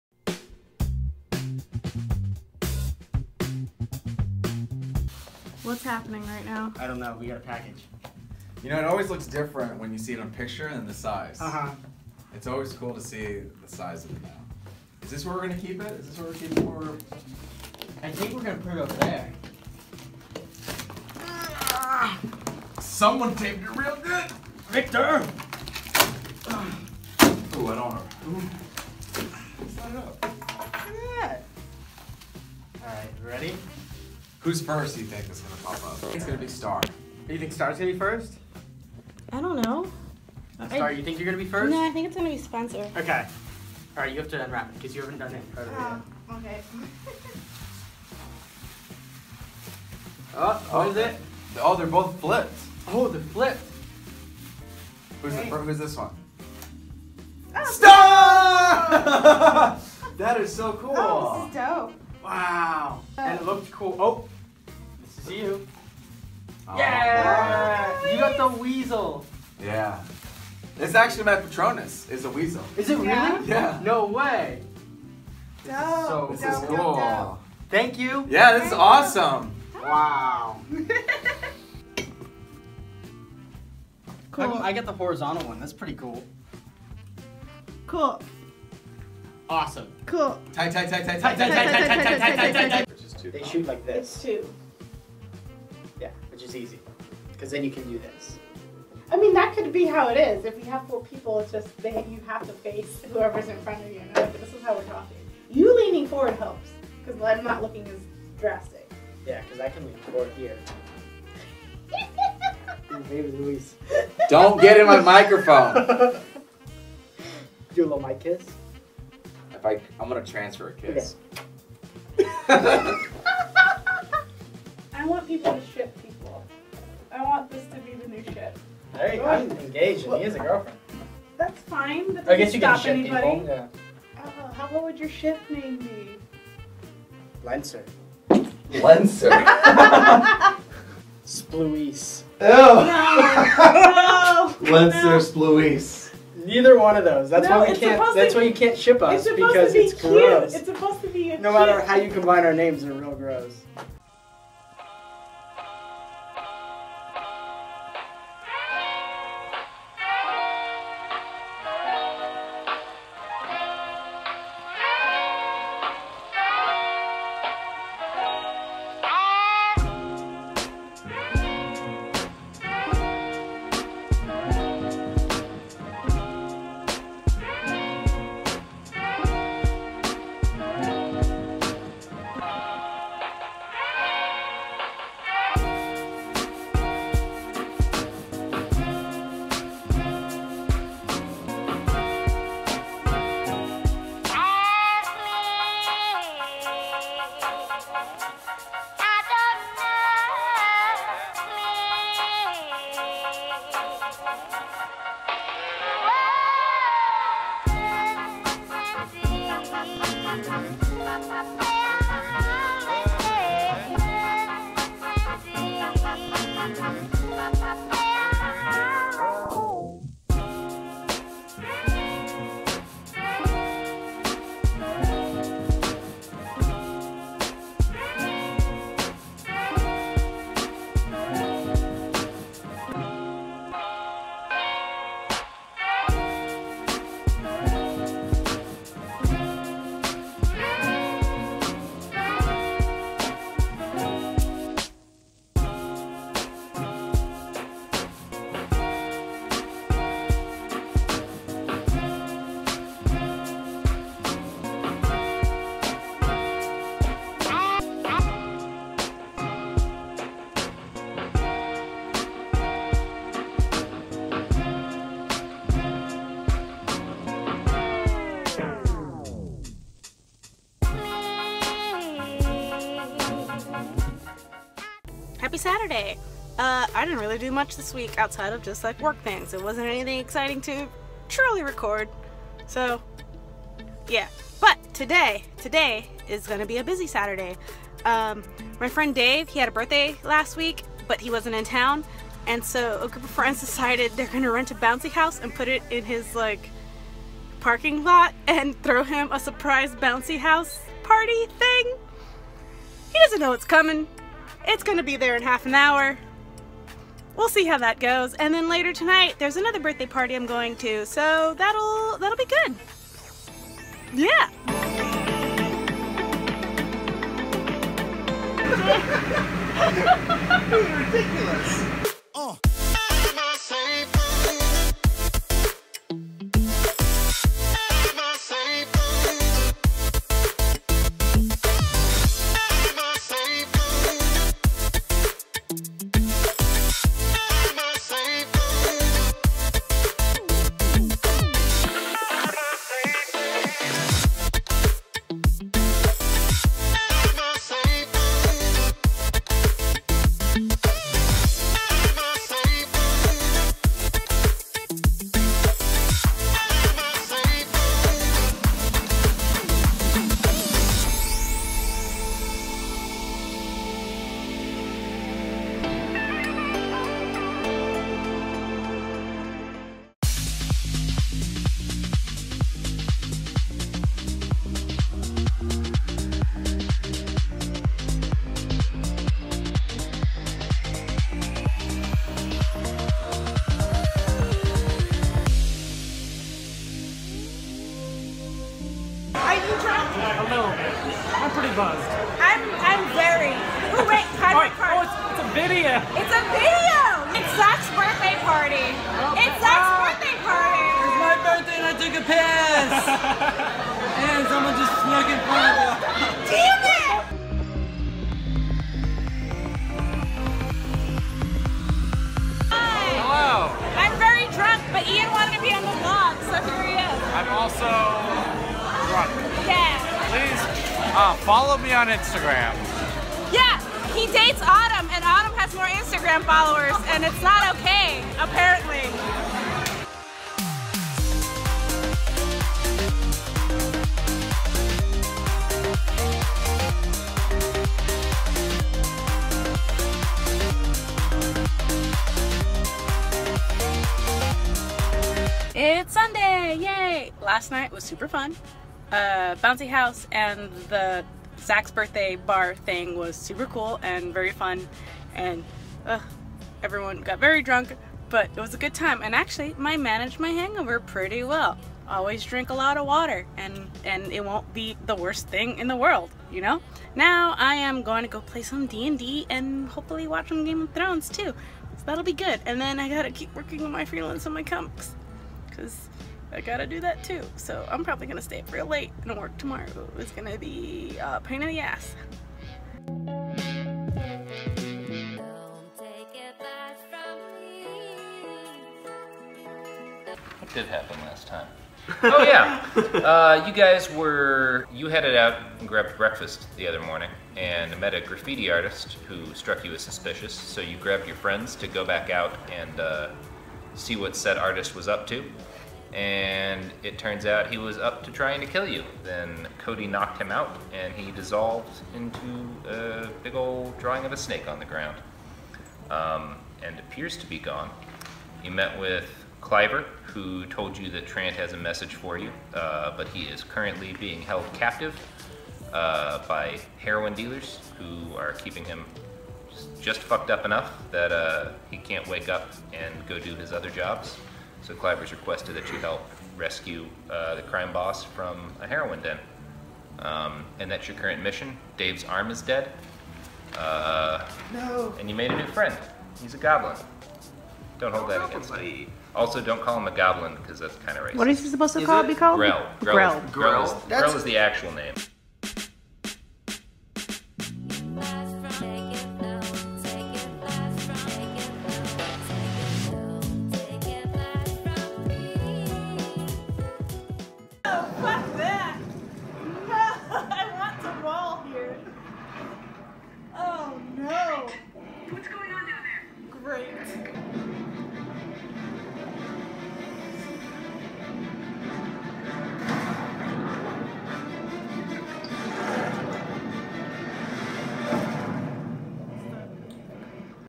Bounce House & Bars - What I Did Last Week - 7 17 17
★ What I Did Last Week of July 17th 2017 ★

- We Stayed Late Got Mail
- Pole Freedance
- Bounce House!
- Zack's Birthday Party
- DnD

♥ Art Wishlist
♥ Silly Wishlist
United States (If you mail something leave a comment about it cause I don't check the box often!)